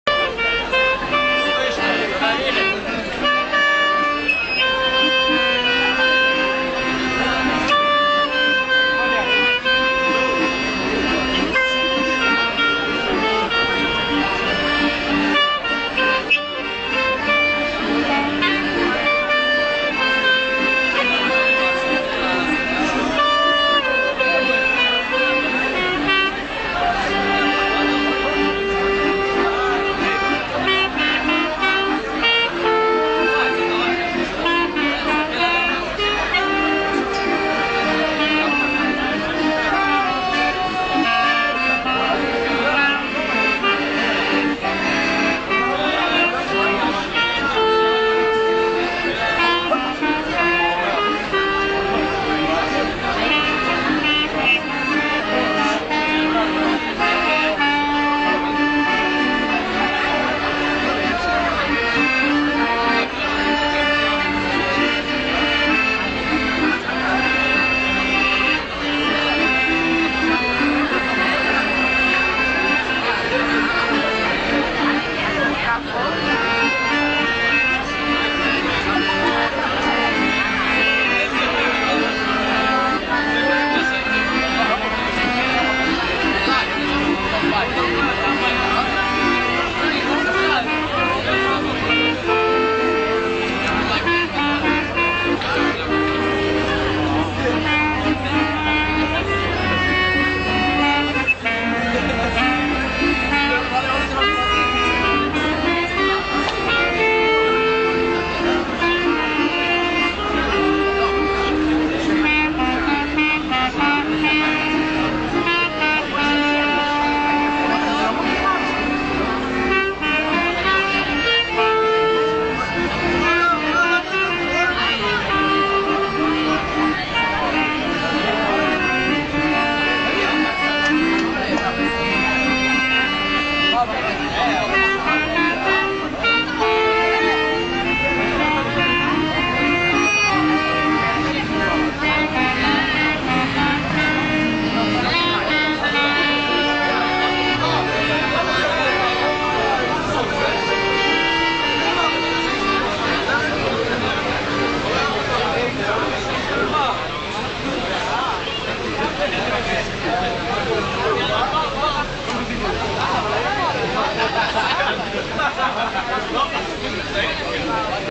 משה איבגי מנגן ברחוב
בכניסה להצגה של "הבן הבכור" בתאטרון גשר
משה איבגי מתחזה לנגן רחוב ומנגן בכניסה בקלרינט
בראיון עימו אמר שלמד לנגן כחלק מהכנה לתפקיד בהצגה